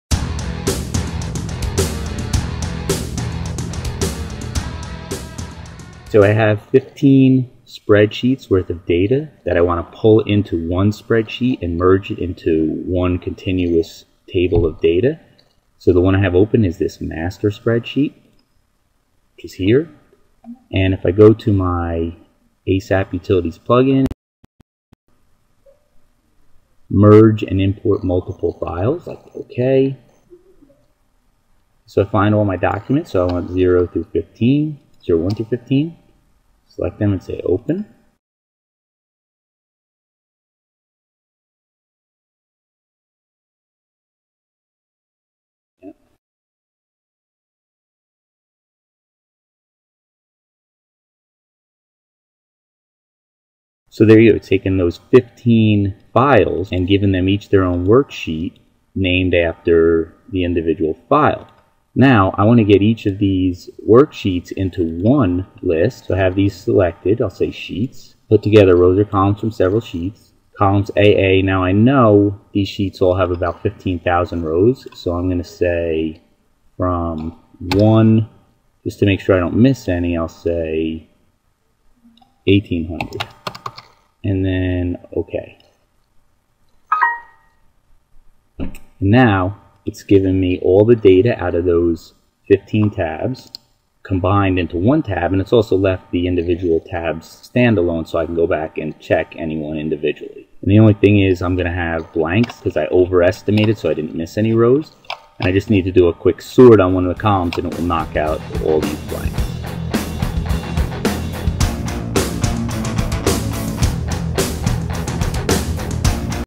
Become an Excel Guru with ASAP Utilities
Learn how to use the ASAP Utilities add-in to dramatically boost the power and ease of using Excel.